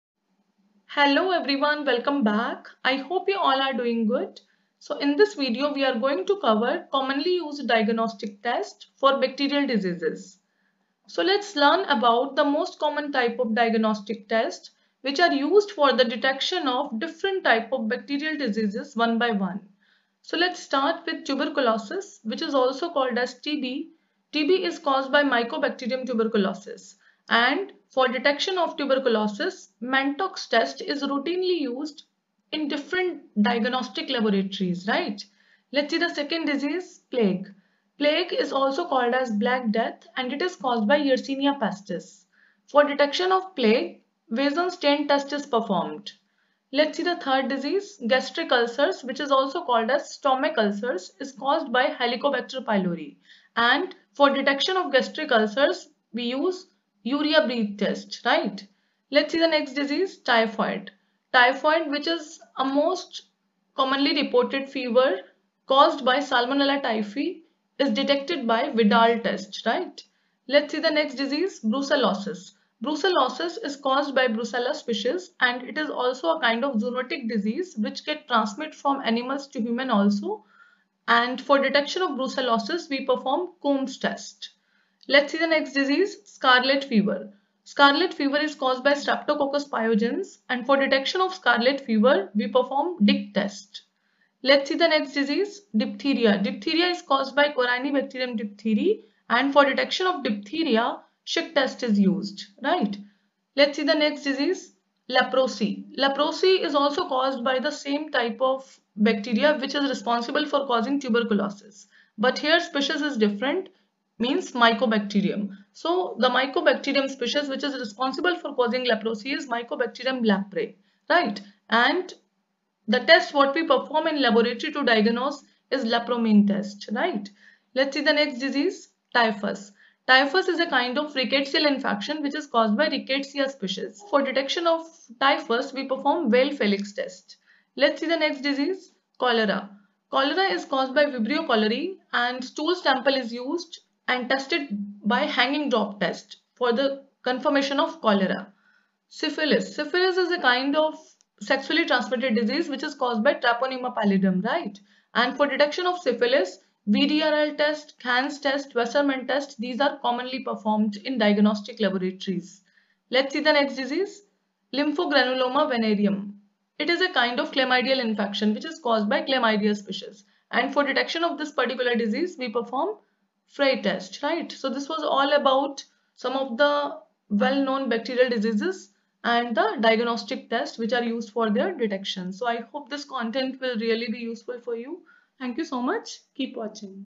Diagnostic Tests for Bacterial Diseases
This video covers most commonly used diagnostic tests for 12 different type of bacterial diseases.